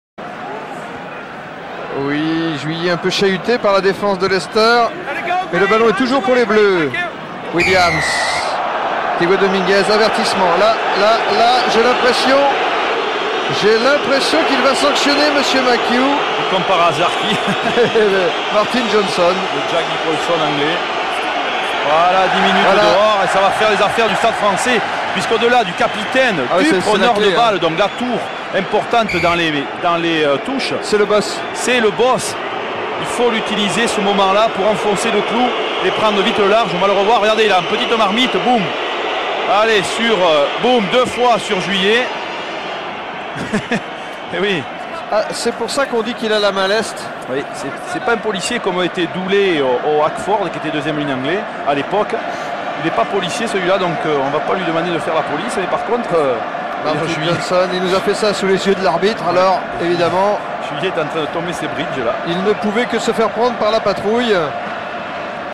Martin Johnson strikes Christophe Juillet in the face
Leicester Tigers captain Martin Johnson is sin binned after punching Stade Français number 8 Christophe Juillet in the face in the 2001 Heineken Cup final after the Frenchman was holding onto his shirt.